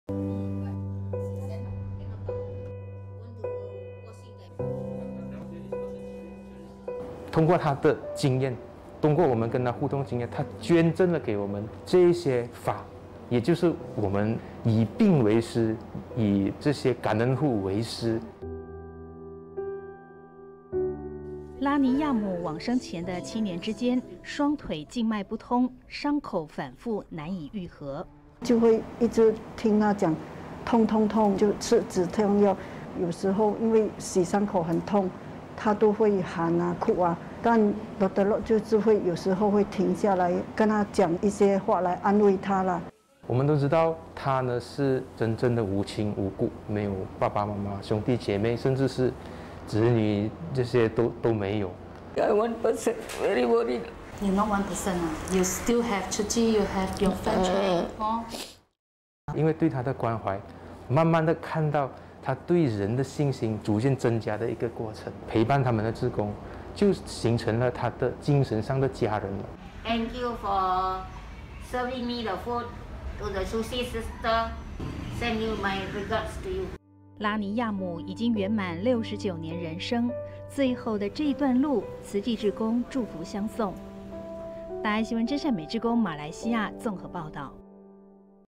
最後一哩路 婦人不孤單
在馬來西亞柔新山，有一位慈濟照顧戶拉妮亞姆，日前病逝，志工與醫師回顧陪伴的7年期間，都說自己也有所成長。

慈濟人醫會醫師 陸忠平：「通過她的經驗，通過我們跟她互動的經驗，她捐贈給我們這一些法，也就是我們以病為師，以這些感恩戶(照顧戶)為師。」

拉妮亞姆往生前的七年之間，雙腿靜脈不通，傷口反覆難癒合。

慈濟志工 沈秀華：「就會一直聽她講痛痛痛，就吃止痛藥，有時候因為洗傷口很痛，她都會喊啊哭啊，但陸(忠平)醫師就有時候會停下來，跟她講一些話來安慰她。」

慈濟人醫會醫師 陸忠平：「我們都知道她是真正的無親無故，沒有爸爸媽媽兄弟姊妹，甚至是子女，這些都沒有。」

已故慈濟照顧戶 拉妮亞姆：「我孤單一個人，很擔心。(妳不孤單，妳還有慈濟，妳有朋友。)」

慈濟人醫會醫師 陸忠平：「因為對她的關懷，慢慢地看到她對人的信心，逐漸增加的一個過程，陪伴她的志工就形成了她精神上的家人了。」

已故慈濟照顧戶 拉妮亞姆：「感謝您提供我食物，感恩慈濟師姊，我衷心地祝福您們。」

拉妮亞姆已經圓滿69年人生，最後這一段路，慈濟志工祝福相送。

真善美志工 綜合報導